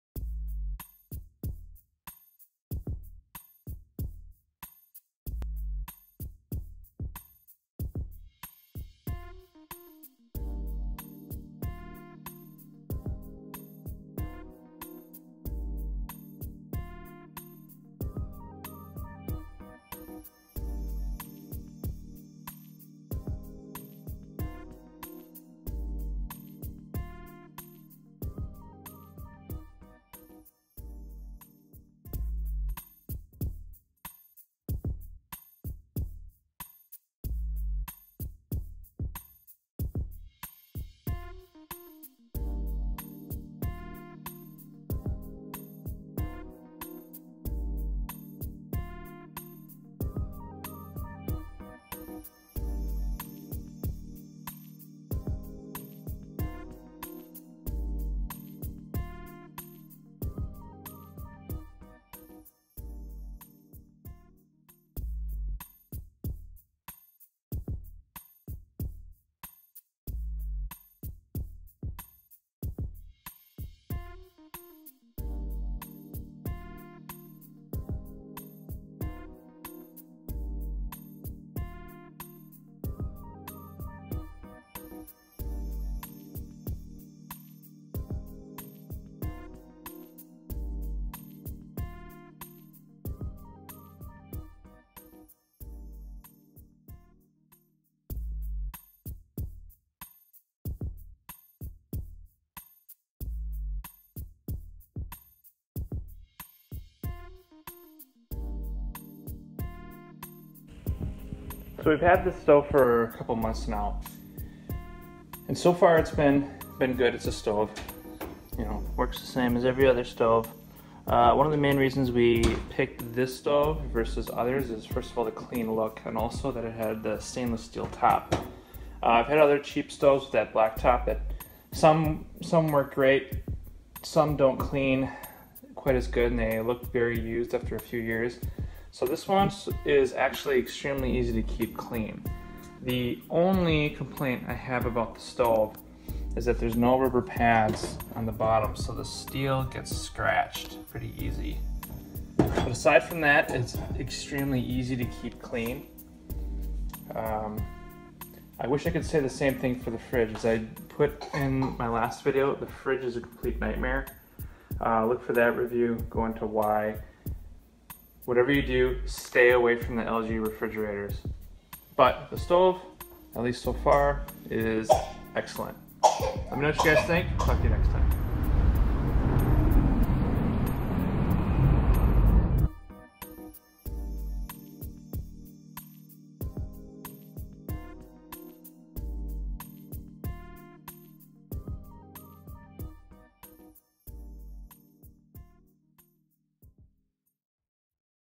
LG stove Unboxing and Review
A quick unboxing and overview of the LG stainless steel stove

Check out Generation's End FREE with Audible Subscription!!!

Check a similar model on amazon but I found the best pricing was a package deal through Best Buy